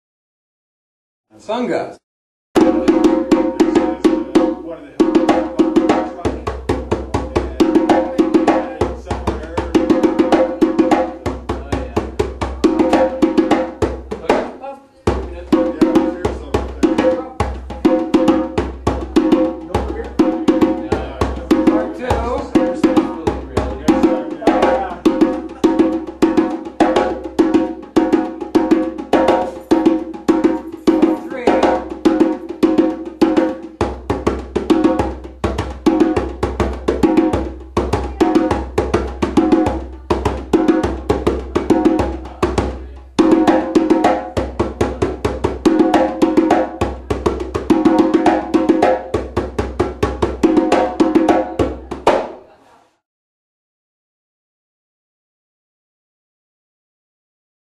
Fanga Hand Drumming Demonstration
African style hand drumming on the djembe featuring the form commonly known as Fanga.